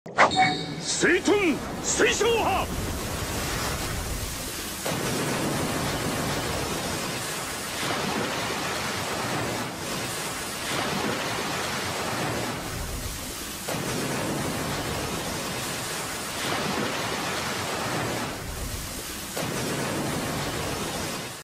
Water Flood on Stationary Vehicle
If you cannot recognize the background audio, then you are not a man of culture. XD

I have posted a first version of this simulation at LinkedIn a month ago. From the comments, I realized I did a mistake that I added unnecessary constraints to the vehicle, thus it only translated in one direction.

This simulation was performed with ALE method with Murnaghan equation of state for water. It was based on the sample file in dynaexmaple and the car was simplified into a rigid body which has no deformable parts to reduce the calculation time. The run time took around 4 hours and 30 minutes.

The original vehicle model and other models can be downloaded online. All the links are provided description of my video:
LS-DYNA TUTORIAL 16: Car Collision - Hatchback vs Pickup Truck